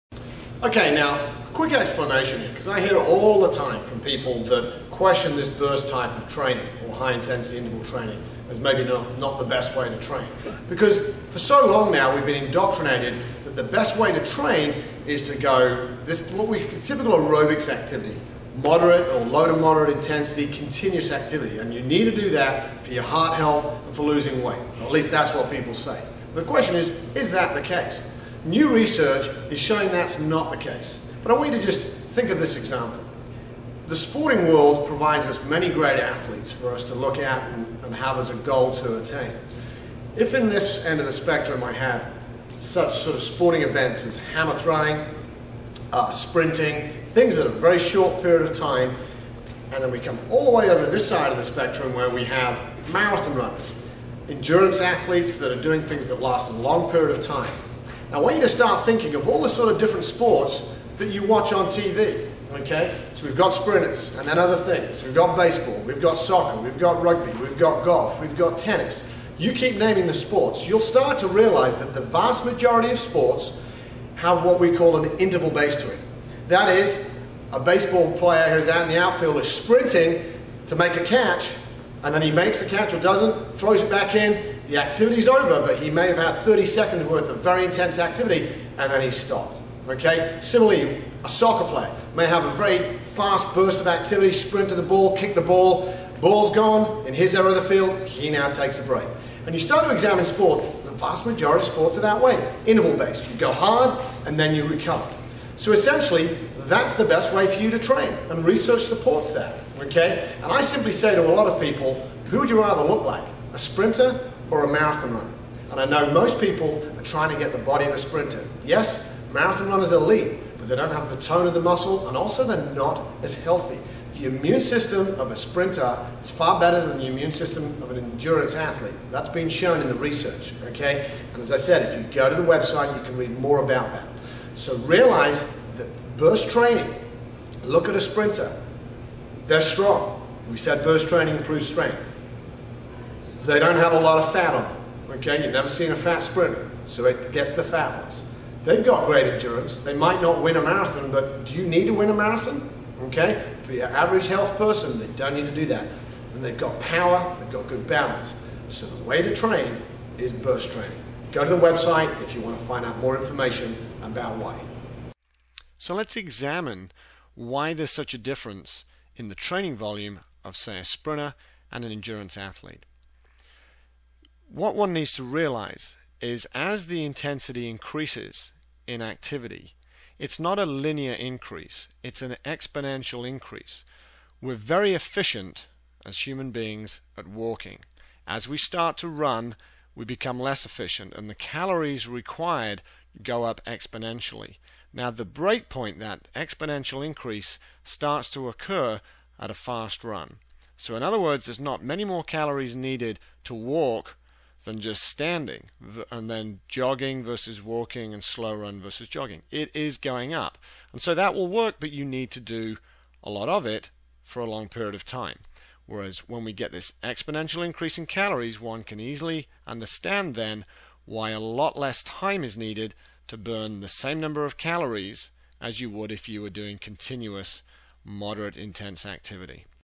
Why High Intensity Interval Training Works
Dr Mark J Smith Explains the benefits of High Intensity Interval Training.